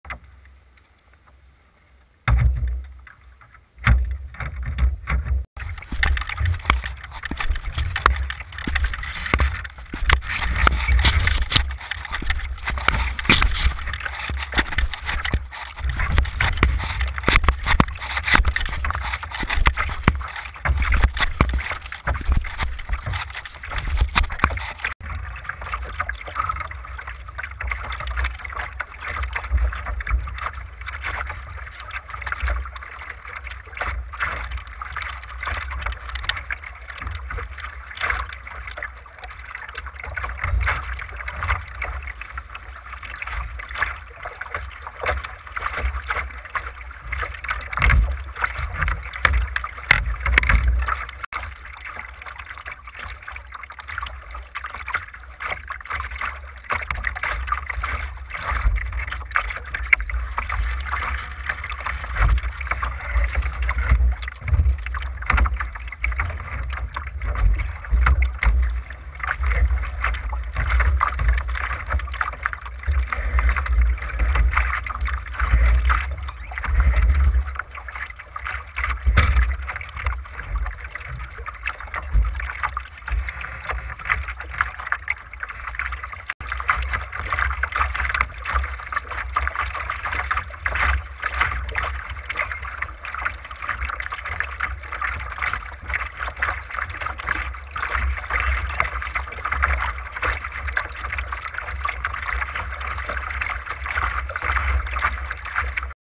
Flying Junior lago d'Orta
Prime uscite con il flying junior dal  circolo vela tramontana